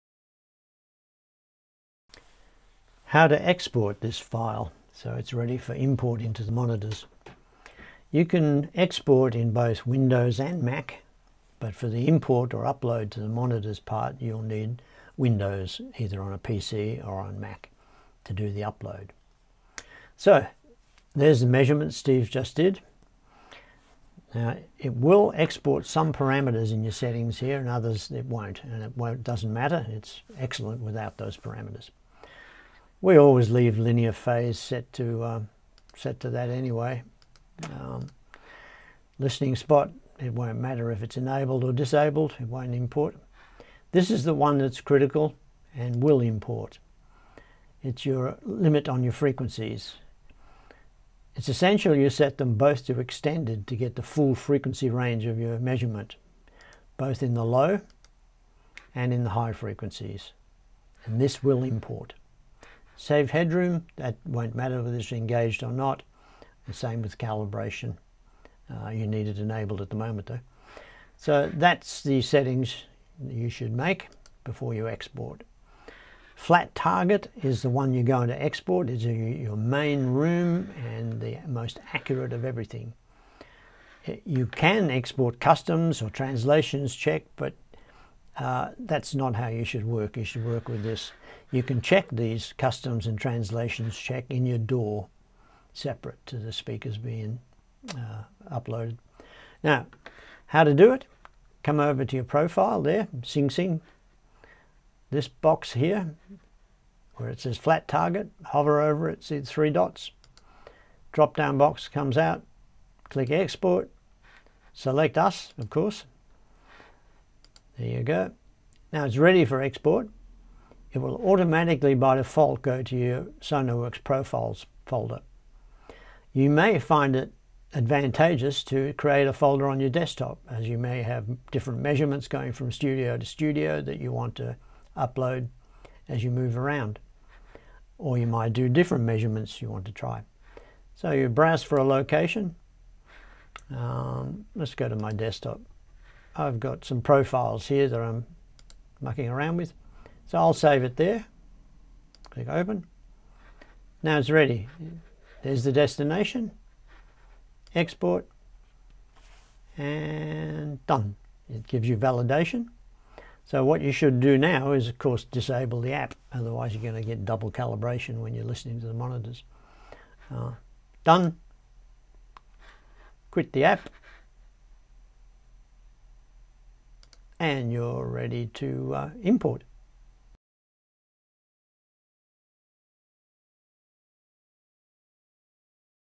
Wayne Jones Audio Monitors - Export SoundID Reference
How to export a Sonarworks SoundID Reference room measurement profile for Wayne Jones Audio (Jones-Scanlon) Studio Monitors.

Sonarworks SoundID Reference profile can now be stored directly within Wayne Jones Audio Studio Monitors